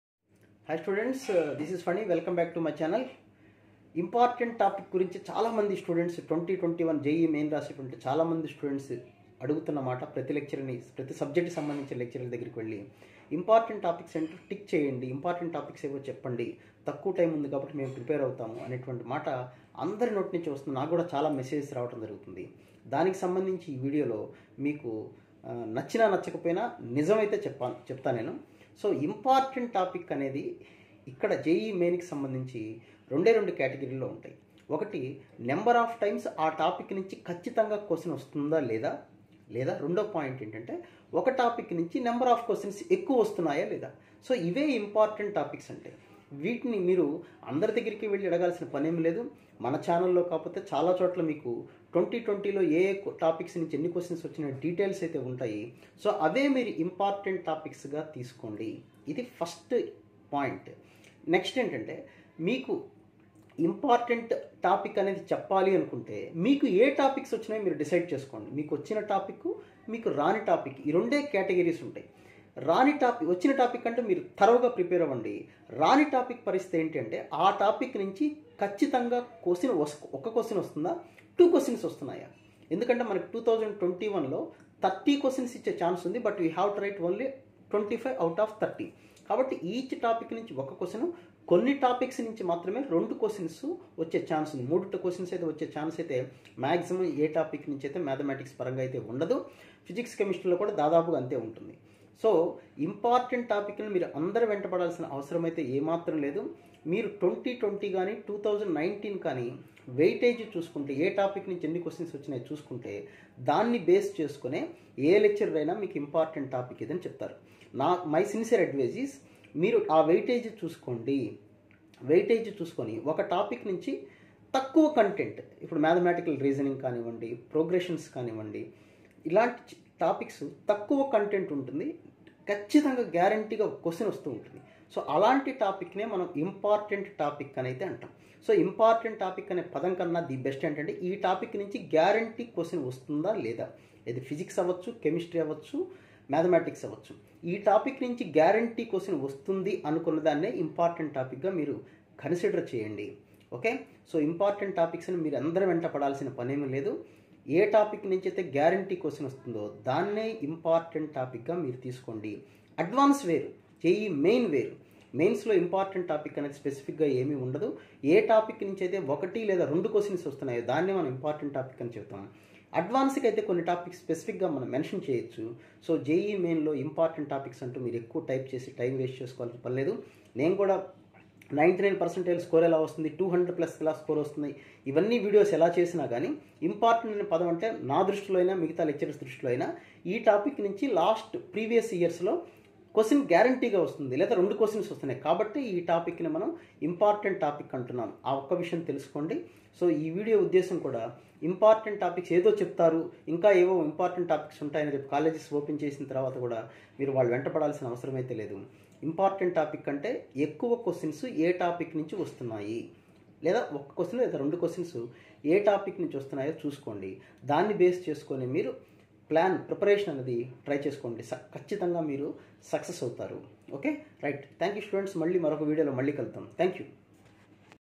Important topics for JEE MAIN 2021# Facts & Truth # In Telugu # PHANI sir #
Hi students in this video am going to clarify to all Jee 2021Aspirants What about Important Topics in Maths, Physics and Chemistry.
   How we identify a topic which is important for JEE MAIN.